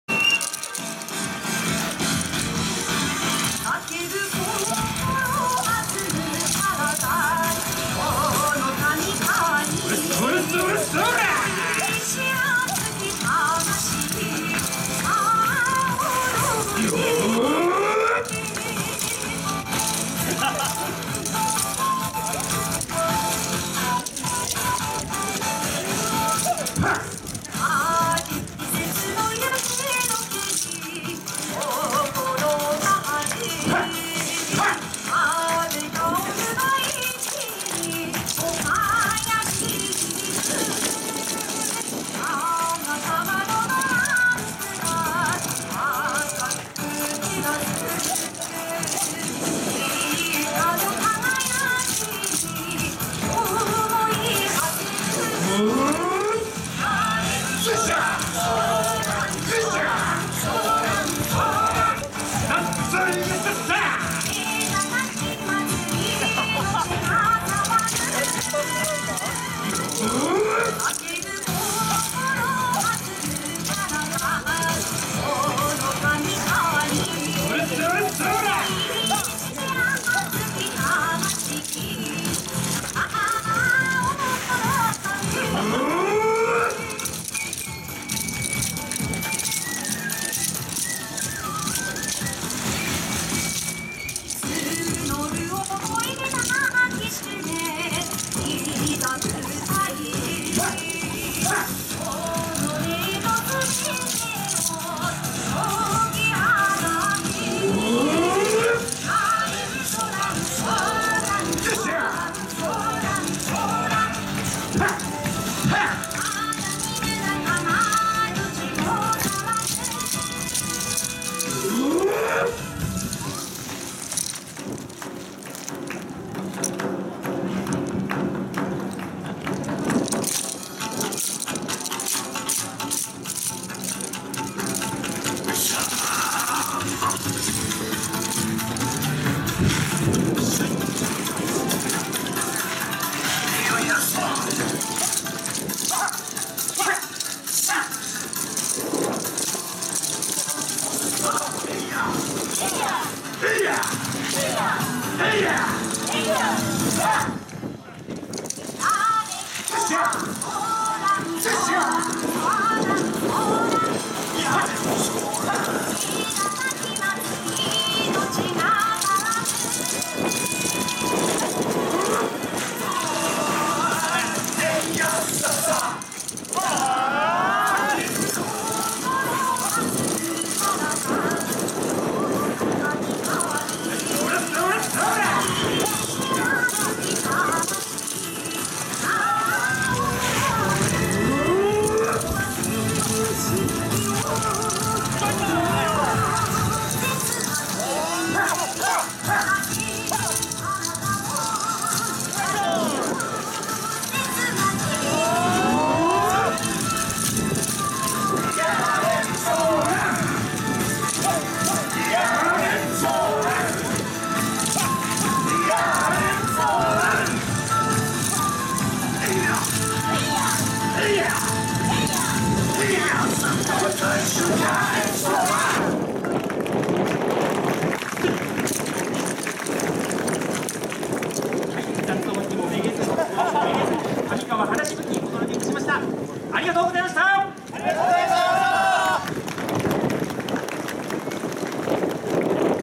道の駅庄和'18 5月午後 大宮同心桜連 - かみかわ華しぶき
18-05-06 よさこい応援団チャリティ演舞
埼玉県 春日部市 庄和 道の駅
強い日差しと強風の中の演舞　一部に音響トラブルがありました